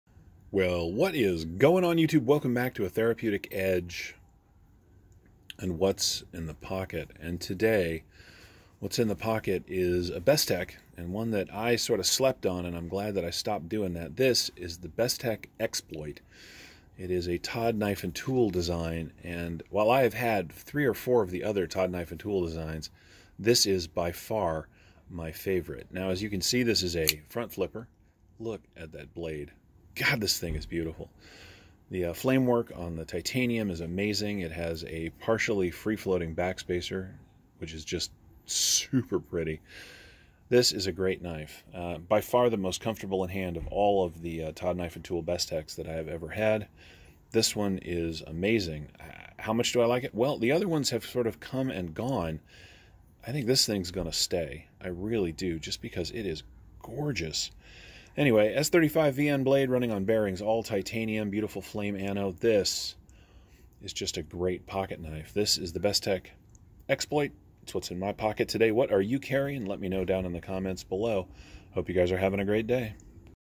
What's in the pocket? (5/16/21)
The TKT designed Bestech Exploit is what's in the pocket today.

What are you carrying?